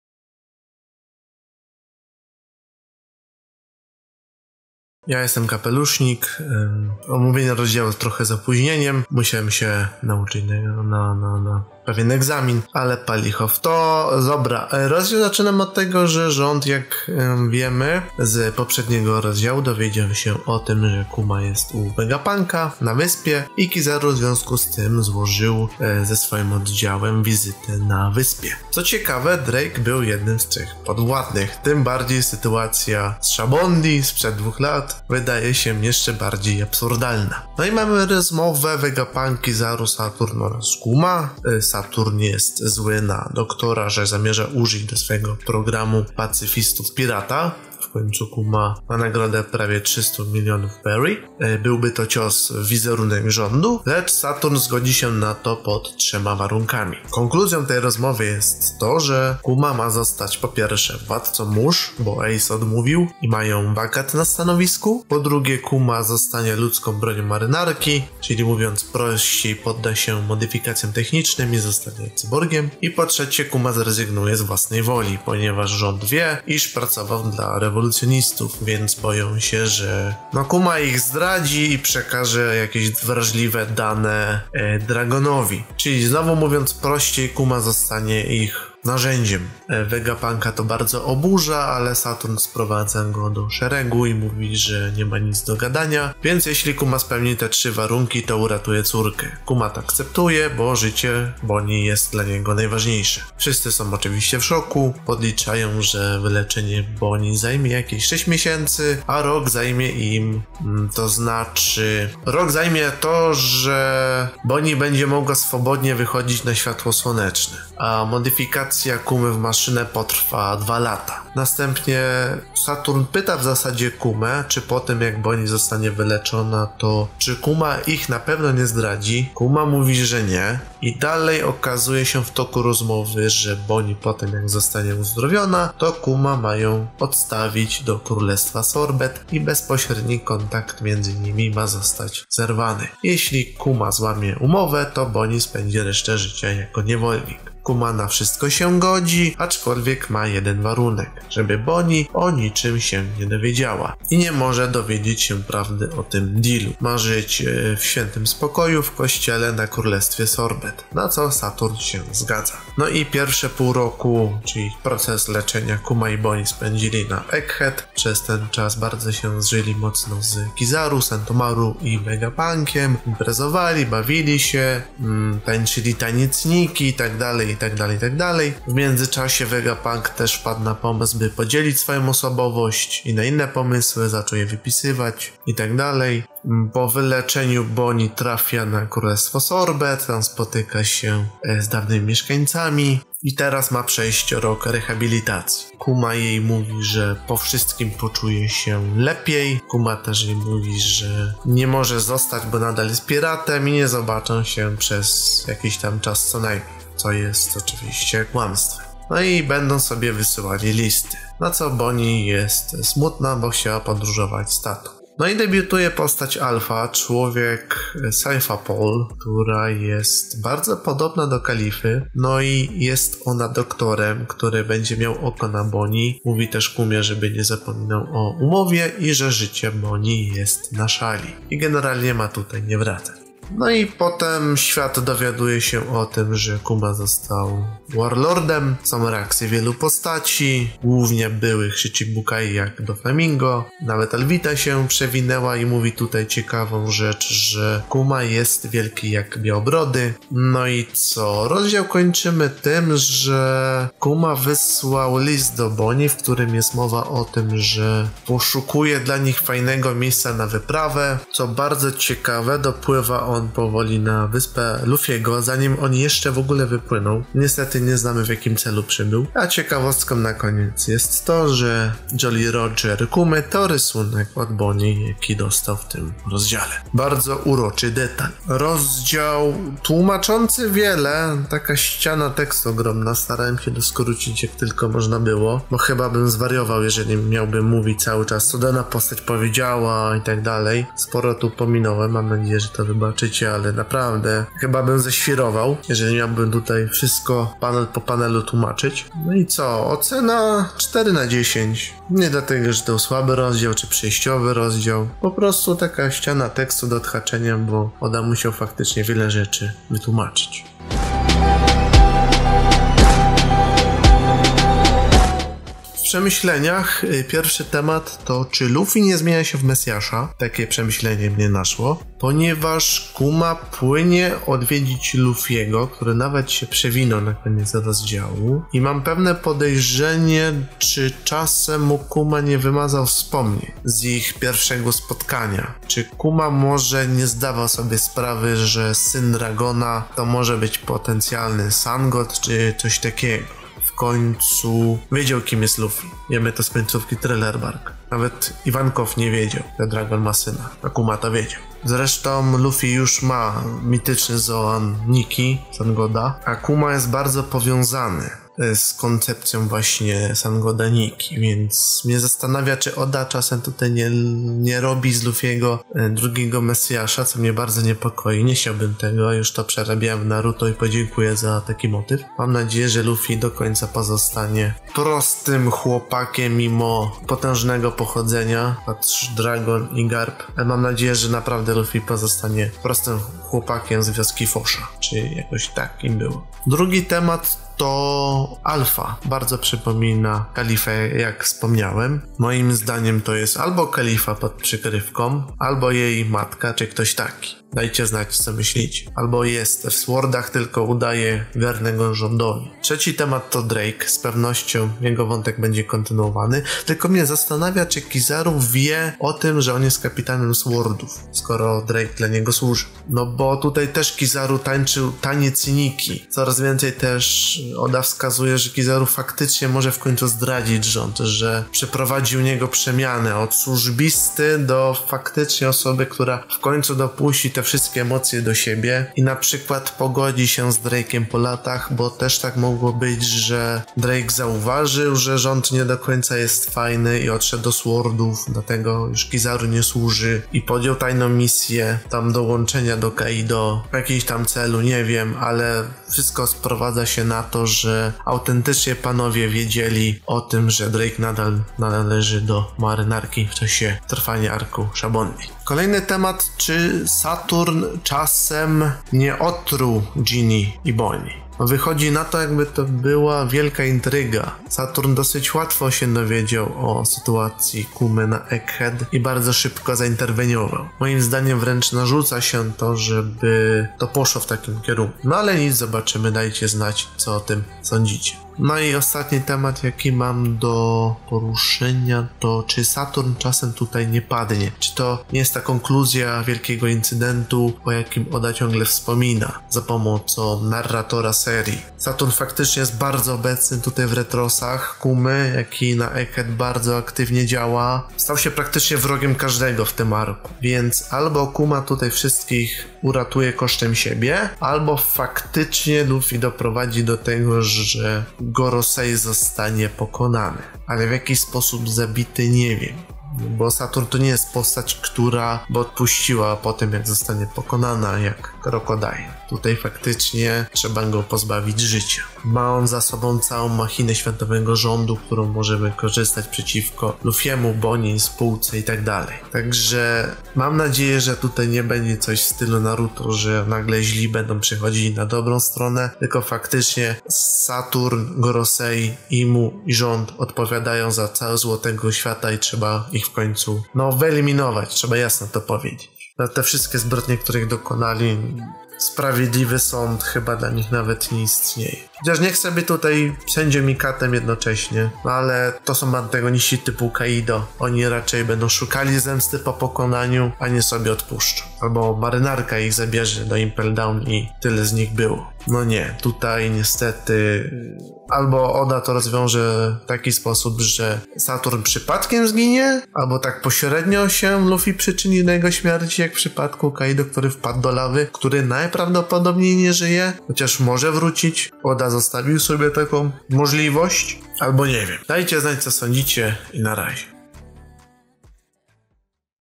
OMÓWIENIE ROZDZIAŁU ONE PIECE #66 ~ DEAL Z SATURNEM (ROZDZIAŁ 1100)
SCENARIUSZ: KAPELUSZNIK & DESTHER
MONTAŻ: KAPELUSZNIK
NARRACJA: KAPELUSZNIK
TWÓRCA MINIATURKI: KAPELUSZNIK

SPIS:

0:00 - 5:11OMÓWIENIE 1100
5:11 - 9:49 PRZEMYŚLENIA